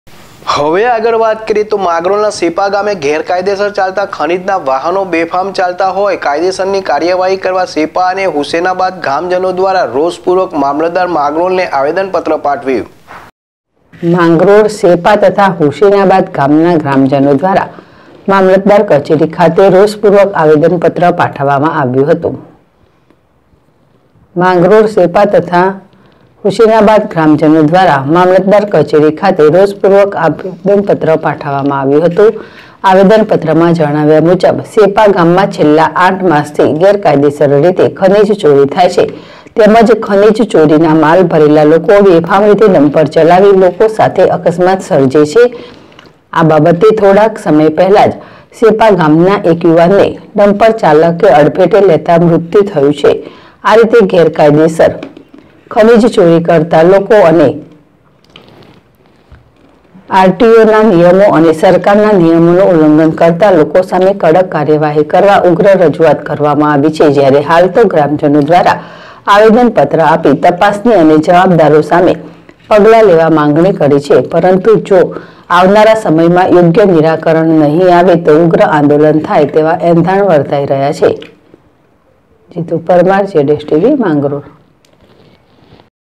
માંગરોળ શેપા તથા હુસેનાબાદ ગામના ગ્રામજનો દ્વારા મામલતદાર કચેરી ખાતે રોષપૂર્વક આવેદનપત્ર પાઠવવામાં
માંગરોળ શેપા તથા હુસેનાબાદ ગામના ગ્રામજનો દ્વારા મામલતદાર કચેરી ખાતે રોષપૂર્વક આવેદનપત્ર પાઠવવામાં આવ્યું હતું